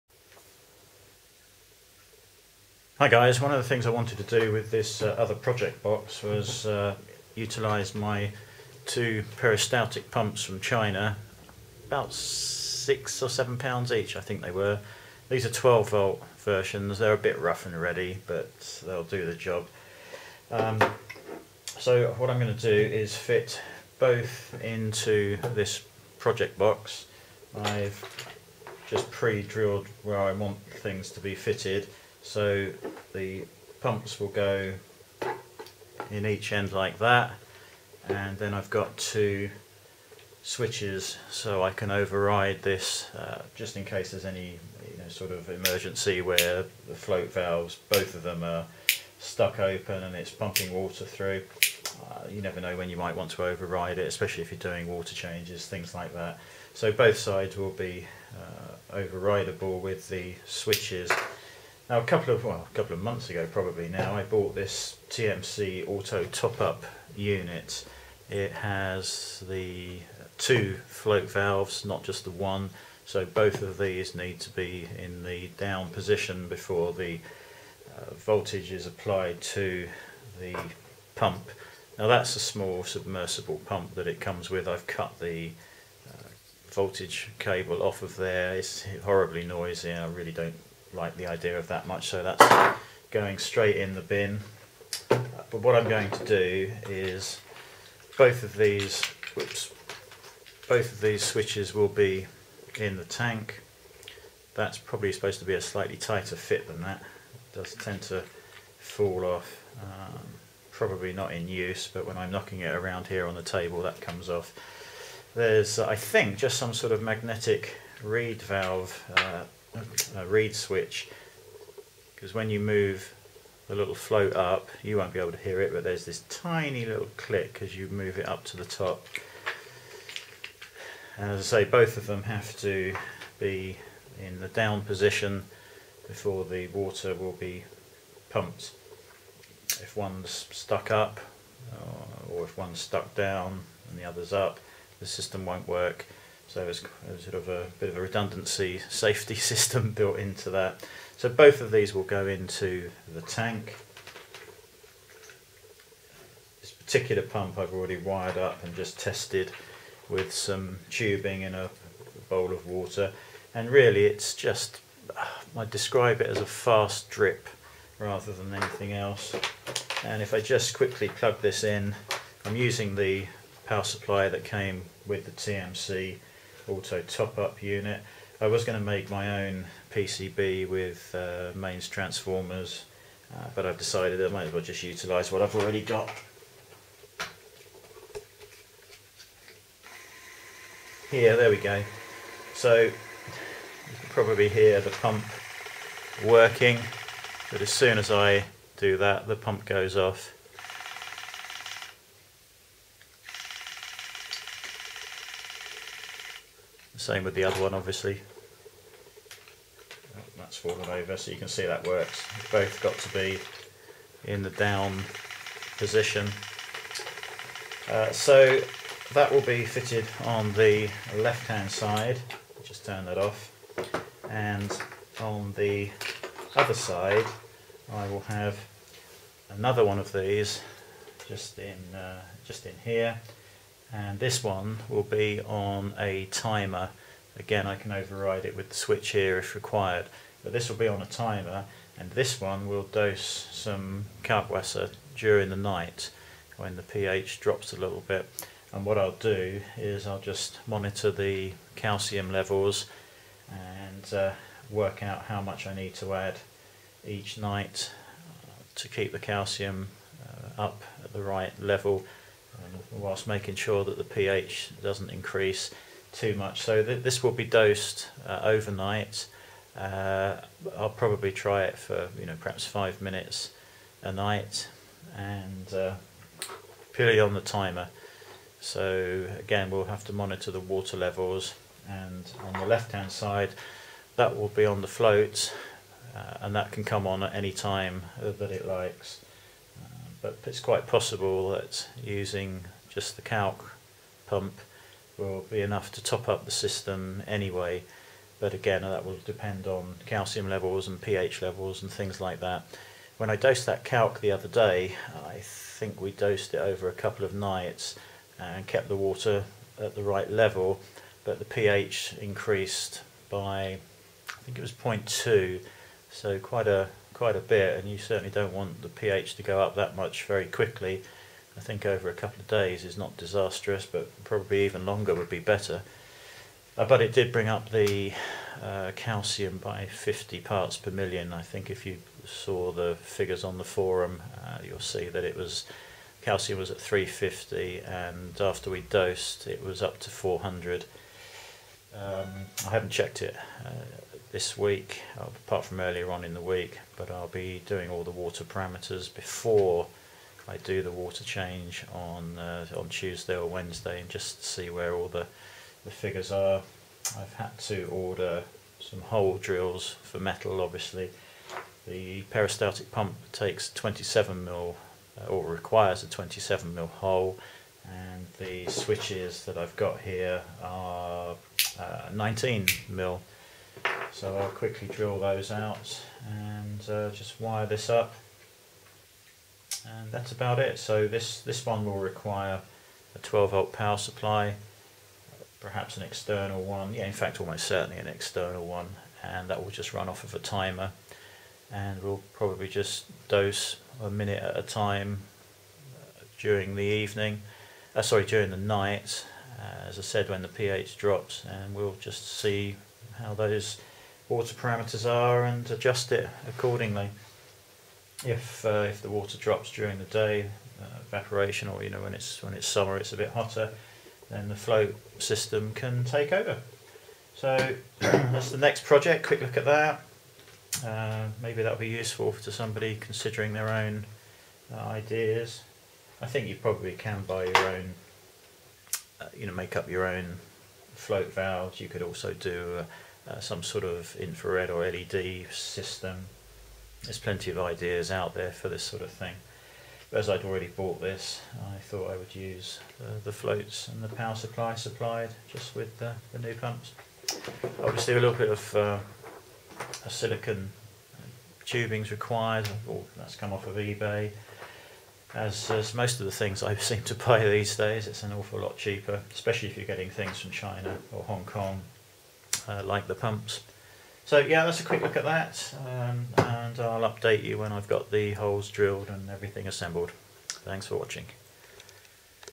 Dual Peristaltic Pump Doser & Top-Off Project. Marine Tank.
A project box and two peristaltic pumps plus TMC's V2 top-off system.
One side working off the floats 24/7 the other side dosing Kalkwasser on a night time only timer.